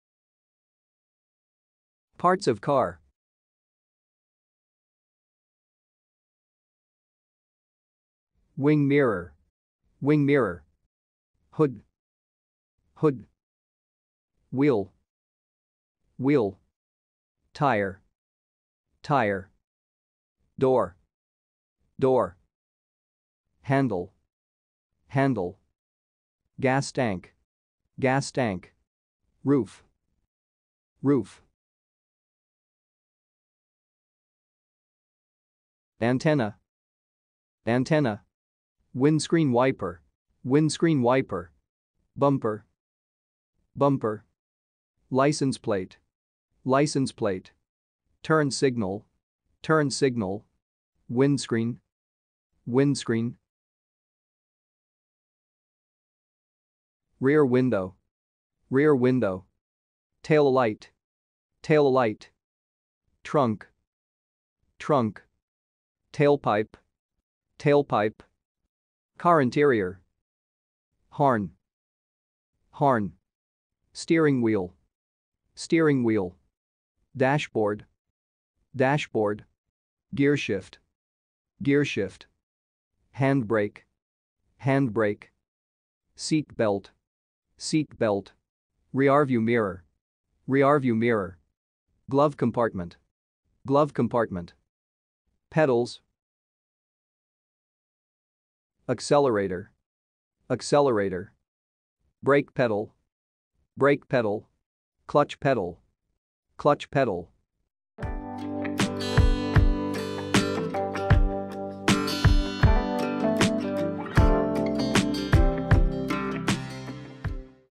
Parts of car in English
00:00 - wing mirror
00:11 - hood
00:14 - wheel
00:17 - tire
00:20 - door
00:23 - handle
00:26 - gas tank
00:29 - roof
00:36 - antenna
00:39 - windscreen wiper
00:42 - bumper
00:45 - license plate
00:48 - turn signal
00:51 - windscreen
00:57 - rear window
01:00 - tail light
01:03 - trunk
01:06 - tailpipe
01:09 - car interior
01:11 - horn
01:14 - steering wheel
01:17 - dashboard
01:20 - gearshift
01:23 - handbrake
01:26 - seat belt
01:29 - rearview mirror
01:32 - glove compartment
01:35 - pedals
01:39 - accelerator
01:42 - brake pedal
01:45 - clutch pedal
🔥Include subtitles with translation in your language🔥
✅Learn new words.
✅Repeat after the speaker.
✅Take the word memory test.